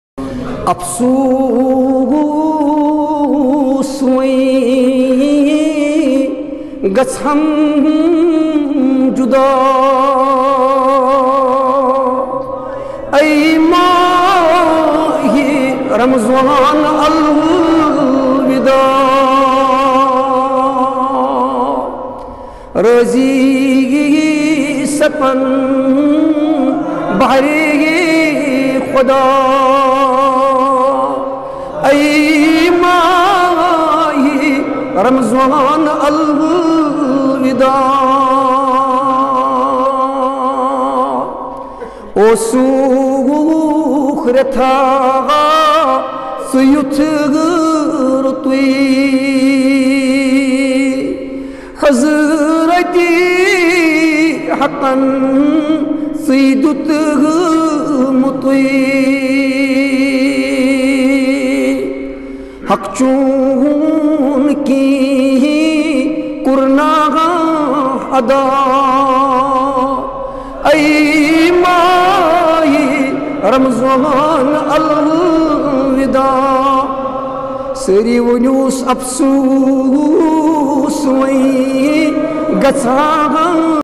Lakhs of people attend Jumat-ul-Vida prayers the last Friday
Lakhs of people attend Jumat-ul-Vida prayers the last Friday of Holy month of Ramadan at Hazratbal shrine in Srinagar

Video by @basitzargarb